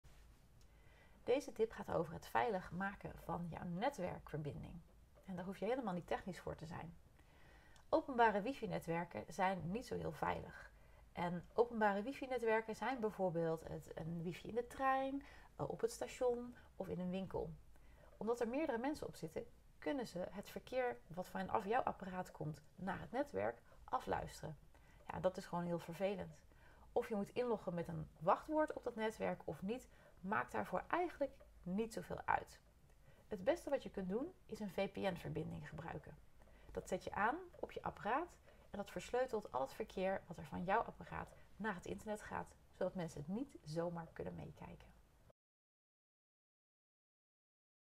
Hoe zorg je voor een veilige verbinding? 🤔
Verbinding maken met een openbaar wifi-netwerk? Helaas kan het gebruik van openbare wifi-netwerken echt een veiligheidsrisico zijn.

Dat zijn wifi-netwerken in de trein in een café of in een hotel. Iedereen in de buurt kan je wifi-signalen gemakkelijk afluisteren met behulp van een standaard laptop of -telefoon en algemeen beschikbare software.

Je kunt hiervoor beter een VPN-verbinding aanzetten, zo kunnen mensen niet zomaar met jou meekijken. Meer weten? Lees verder